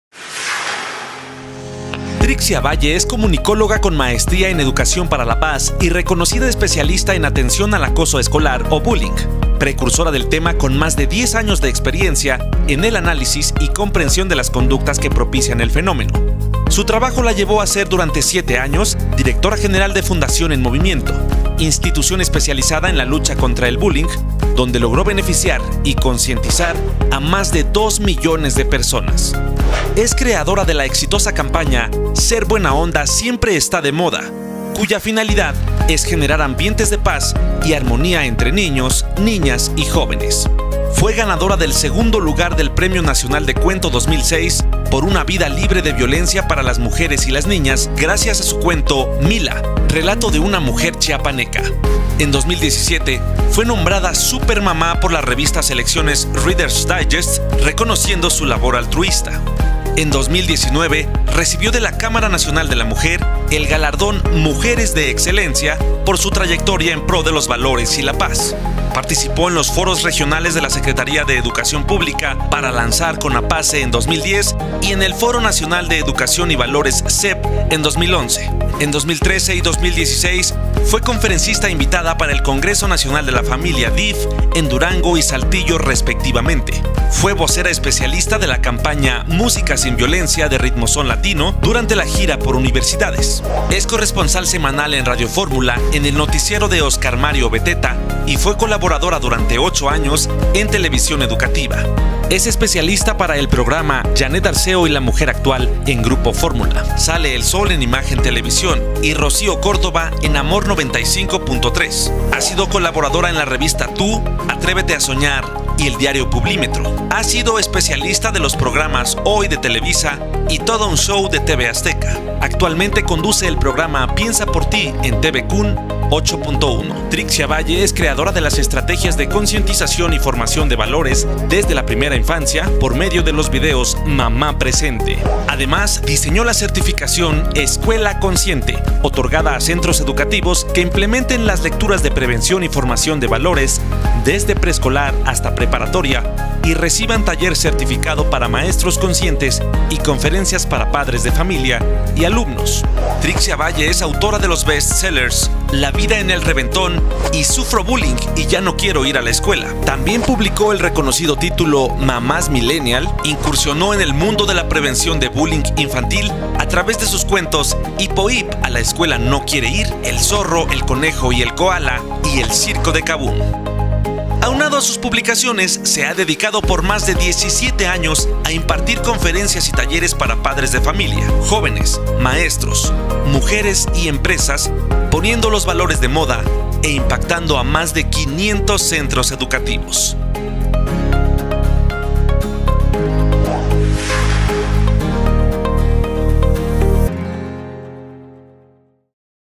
¡Conoce a nuestra escritora: Trixia Valle!
Trixia Valle es comunicóloga con maestría en educación para la paz, actualmente conduce el programa "Piensa por ti" transmitido los martes a las 22:30 hrs a través de SIPSE TVCun (Canal 8.1) Quintana Roo y es una escritora del equipo Timón Educación.